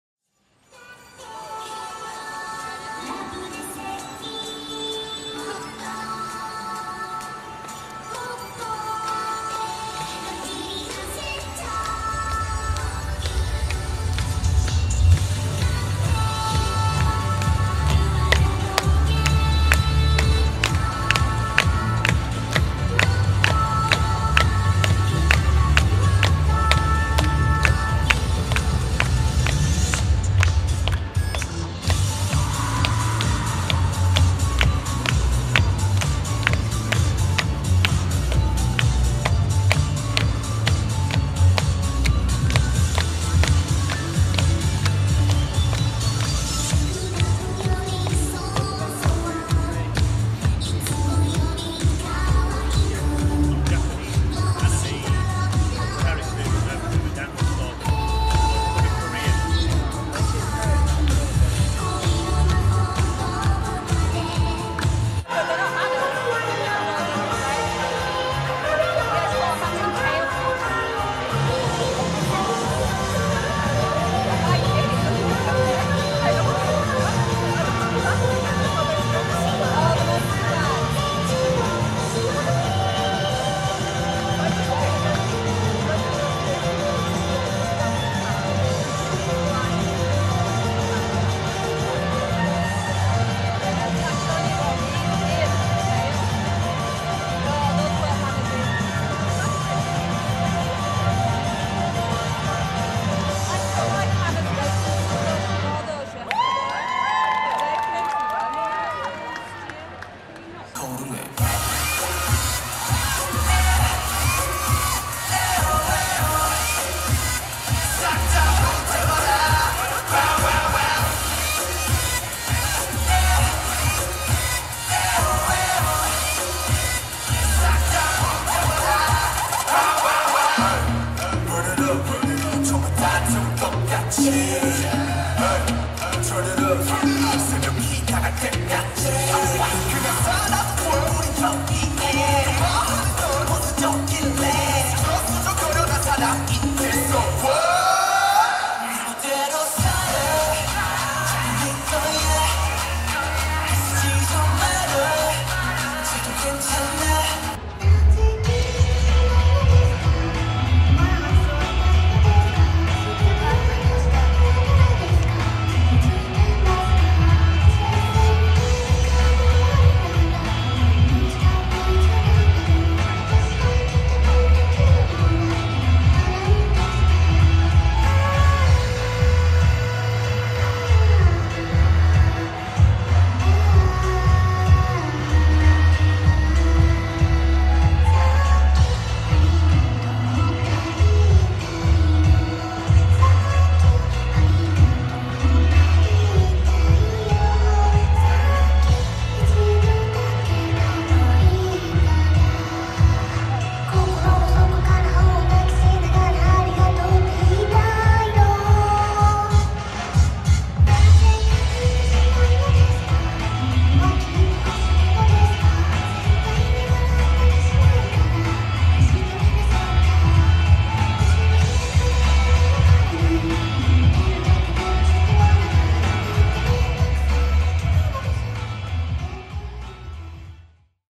Yorkshire Cosplay Con 2017 - Hanami Maid Café and others (Main stage excerpts)
Brief footage I took of Hanami Maid Café (0:00-1:06  and 2:38-3:51) also a solo female dancer and an all male dance group (please comment if you know either of their names) on the YCC main stage.